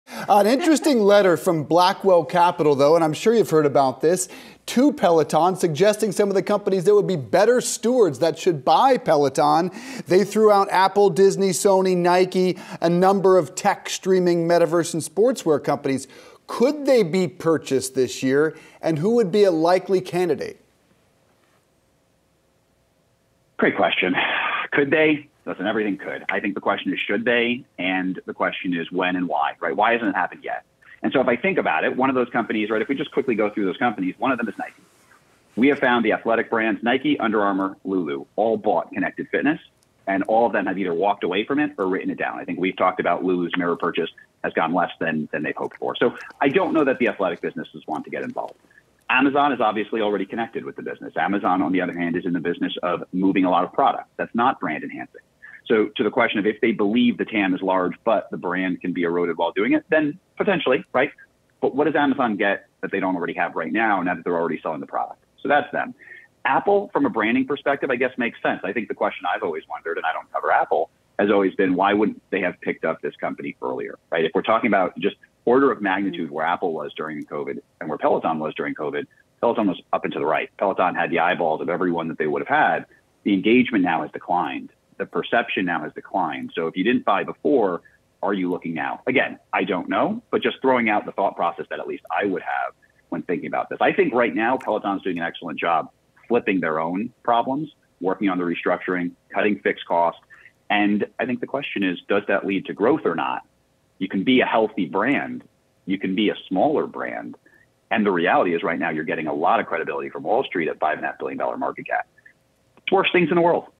Apple could be best brand to buy Peloton: BMO Managing Director:
Simeon Siegel, BMO Managing Director joins Yahoo Finance anchors Seana Smith and Dave Briggs about  Peloton's business and reports of its possible sale to another company. You can see the full interview here. Key video highlights 00:34 On companies that shouldn't buy Peloton 01:06 On Apple's position to buy 01:33 On Peloton's brand